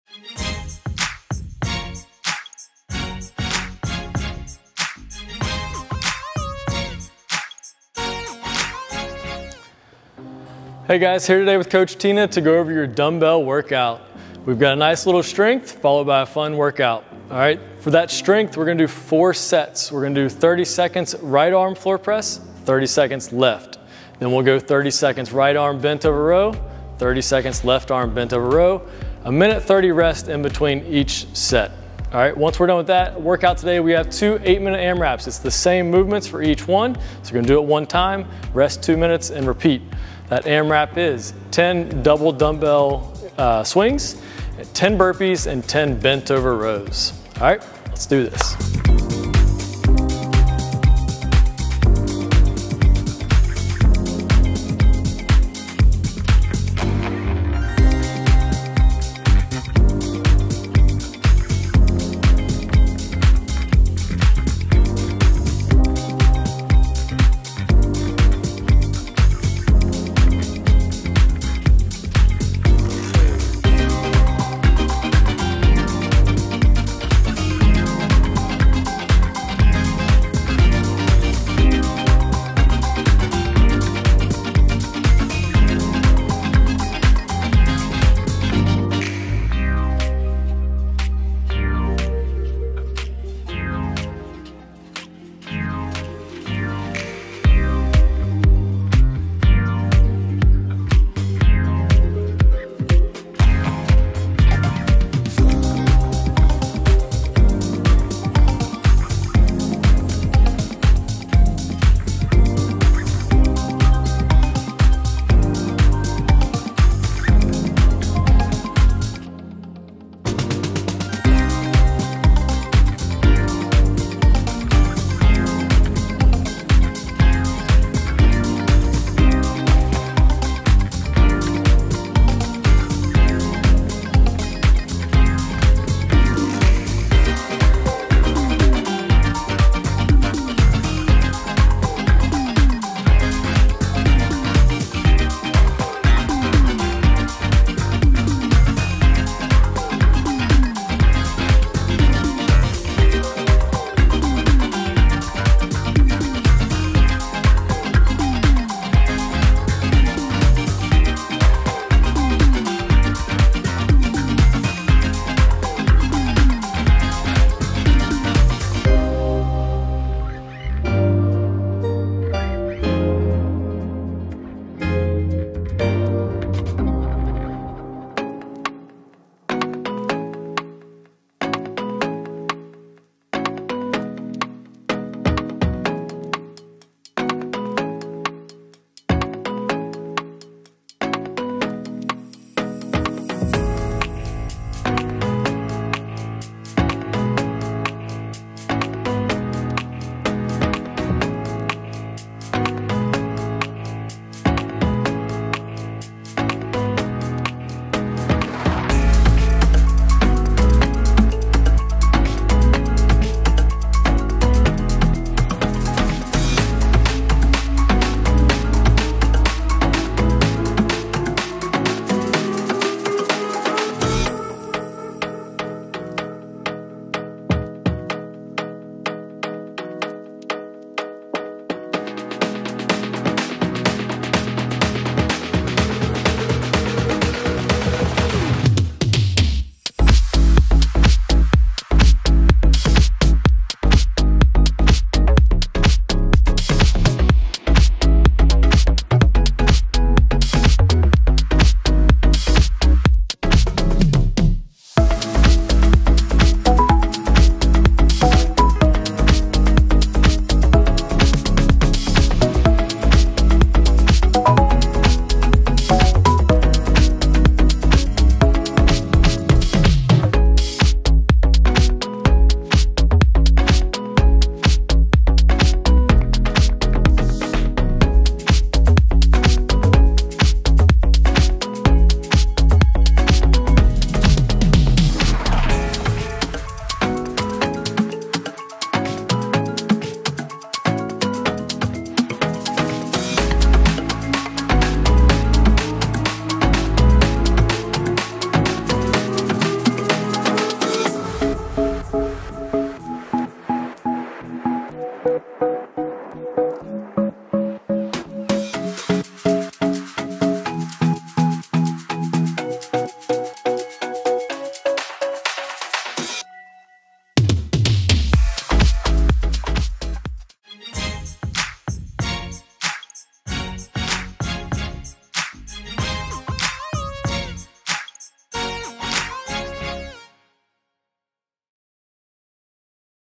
Home Dumbbell Workout | 4-14-20
Home Dumbell workout with Single Arm Floor Press & Single Arm Bent Over Rows as a Strength. Followed by a Couple of AMRAPS

Visit our Gym at:
1010 N Main St